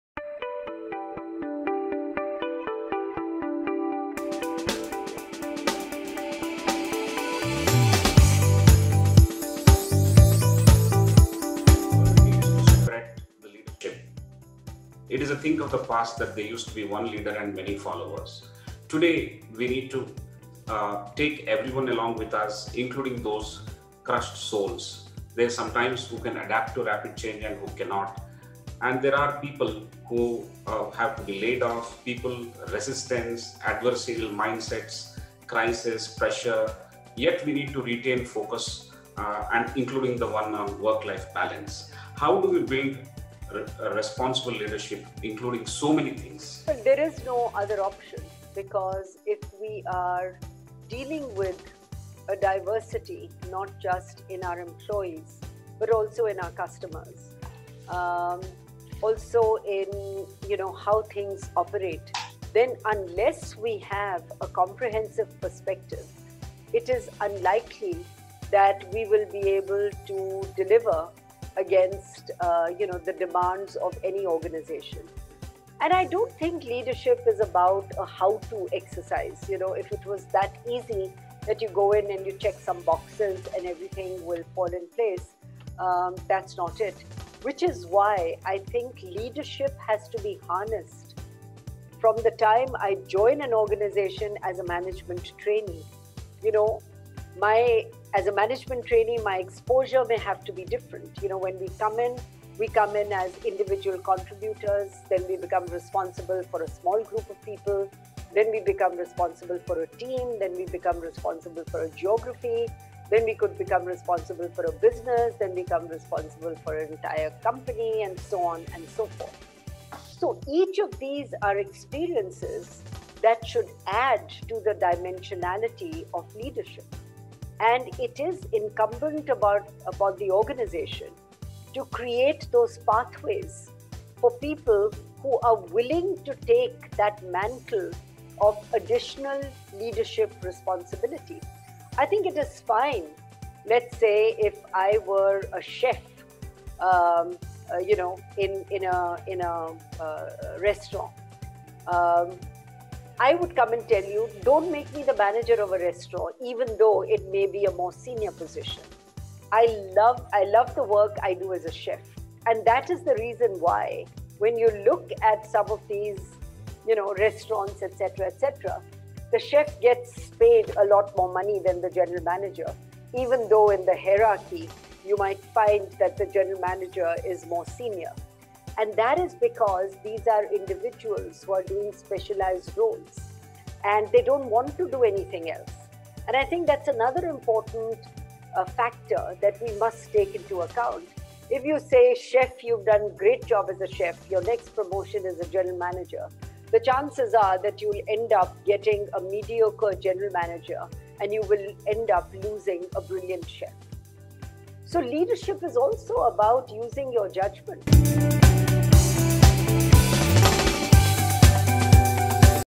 How does the Responsible Leadership Take Care of Complex Situations? | Ms Vinita Bali | ICFAI Online
WiseViews from Industry Leaders.